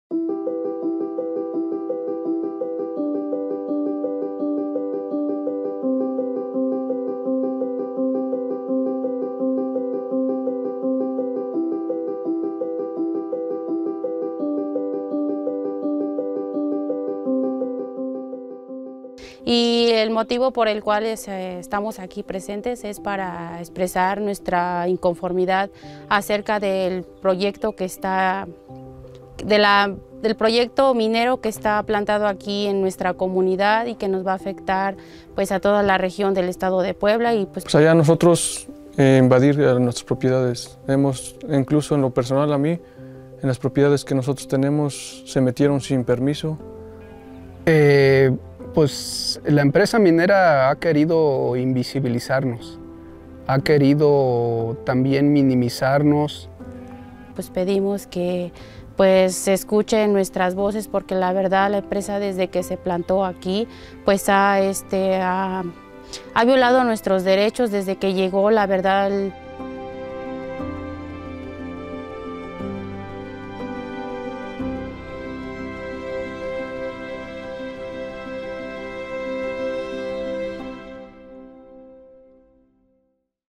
Stories from Mexico: "We Want Our Voices to be Heard"
Watch our one-minute explainer video to understand the importance of telling the stories of communities rendered invisible under the power-hungry agenda of a mining company focused on depleting their resources without their consent.

A project of International Corporate Accountability Roundtable in collaboration with Project on Organization, Development, Education and Research (PODER).